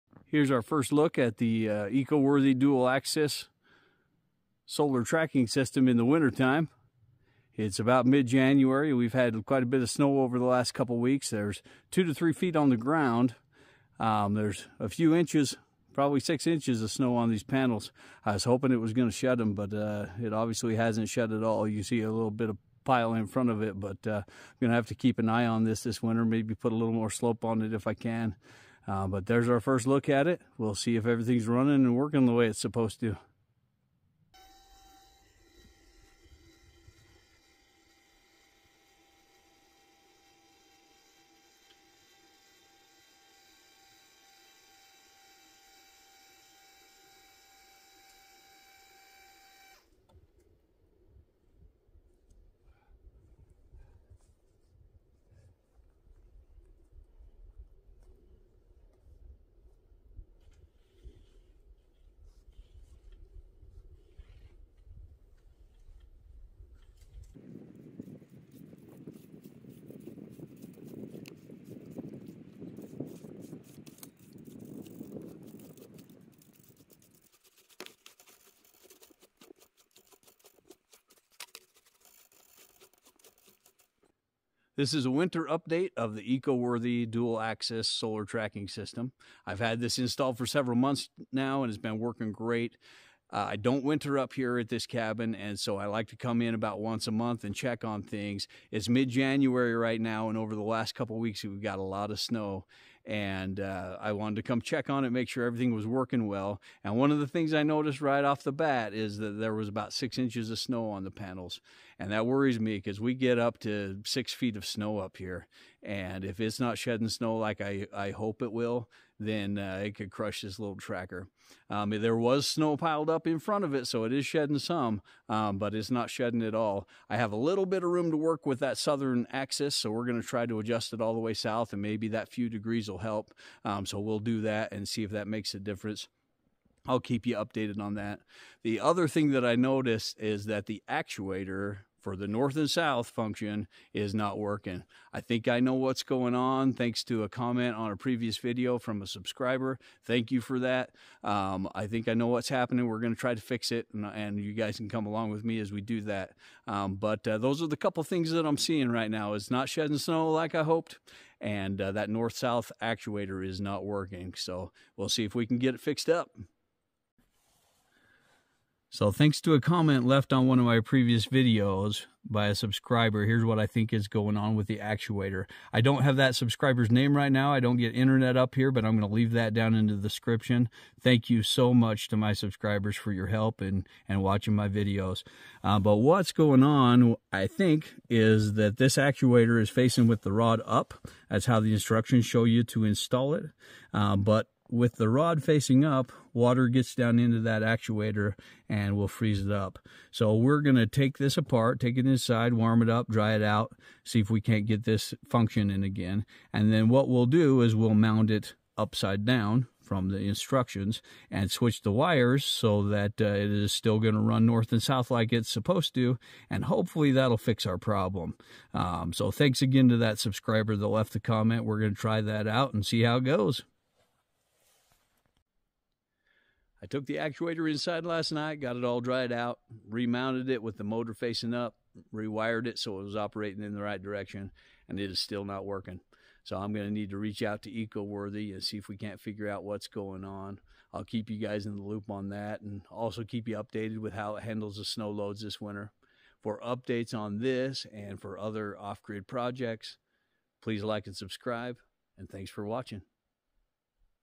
Eco-Worthy Dual Axis Solar Tracker - Winter Update & Review
Winter update on the Eco-Worthy Dual Axis Solar Tracking System Post. Thank you to Sam for your comment on my previous review video and the information about the actuators.

***UPDATE: Eco-Worthy reached out to  me within 24 hours of contacting them with a fast solution. The replacement actuator arrived within 3 days.  So far I am impressed by their customer service. The new actuator has been installed and the tracker is working again.***

I was not compensated for this video in any way. I purchased this tracking system and the content of this video is my honest opinion.

Eco-Worthy Dual Axis Solar Tracking System

Solar Isolator Switch

Quality Solar Cables 10AWG

Awesome MC4 Connector Tool Kit👍🏻